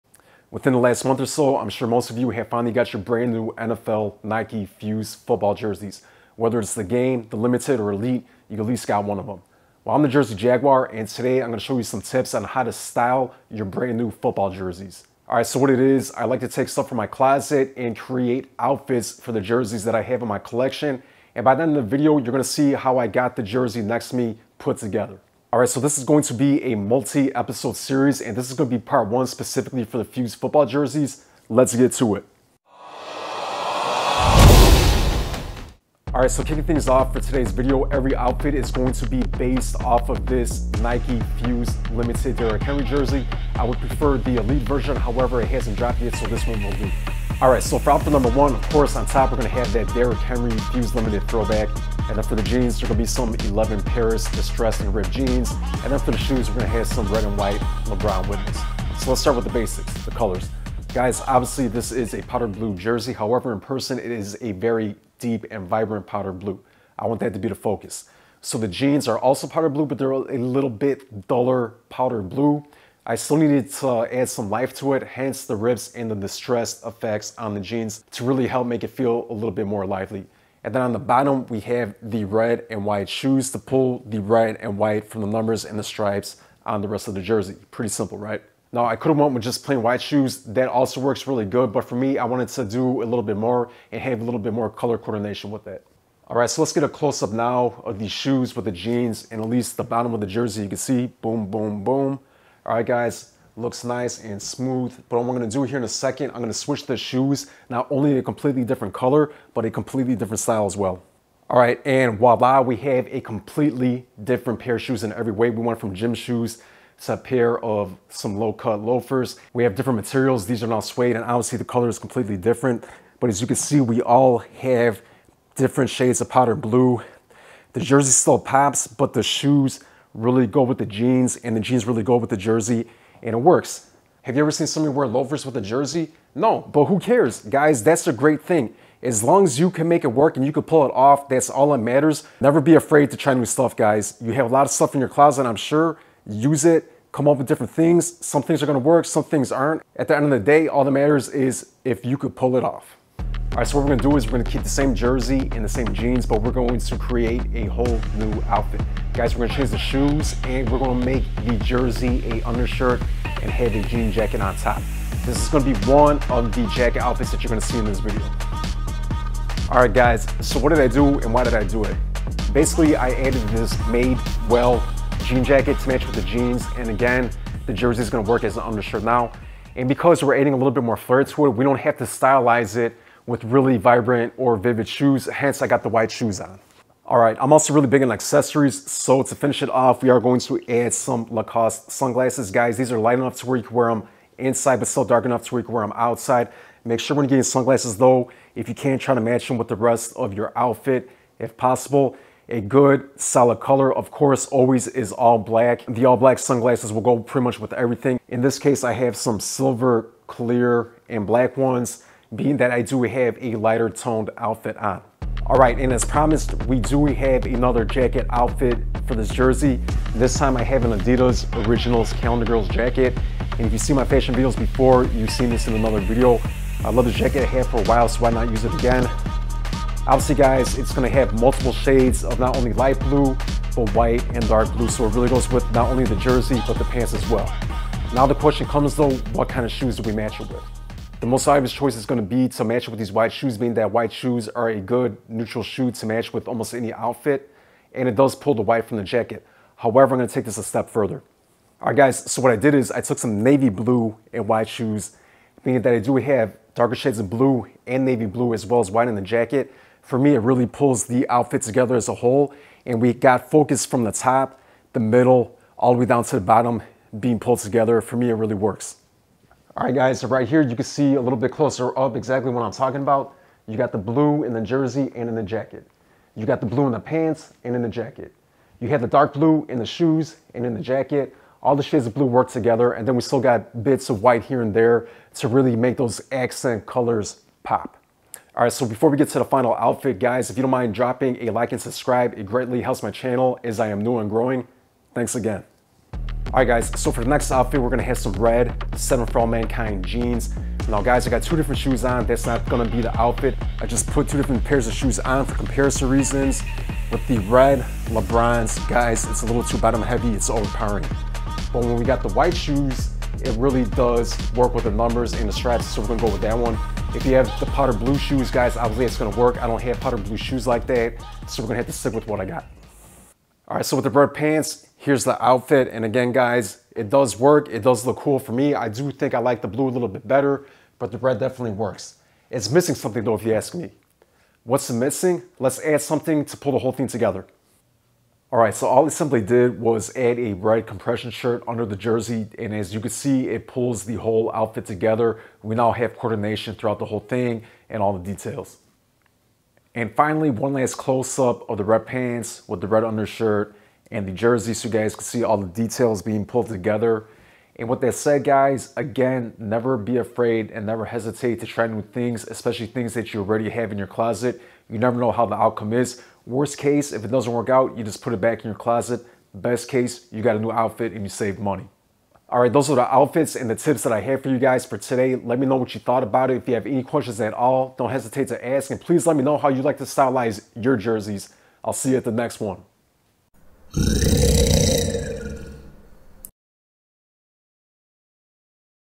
How to Style a NFL Nike FUSE Jersey - Part 1
Today I take my fashion and modeling experience and create a few outfits with tips your your new NFL Nike F.U.S.E Jerseys.  All the items are stuff I pulled from the closet. How do you style your Nike FUSE Jerseys?